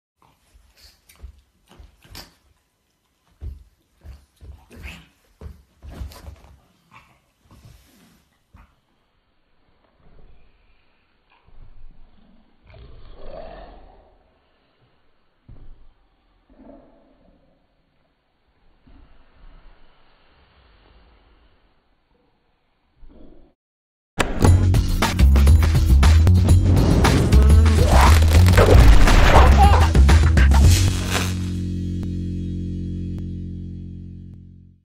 Dramatic pug overreacts over stolen bone
Sanka the French Bulldog was just innocently chewing on Enkei's bone when Enkei decided to throw the funniest temper tantrum ever! Hilarious!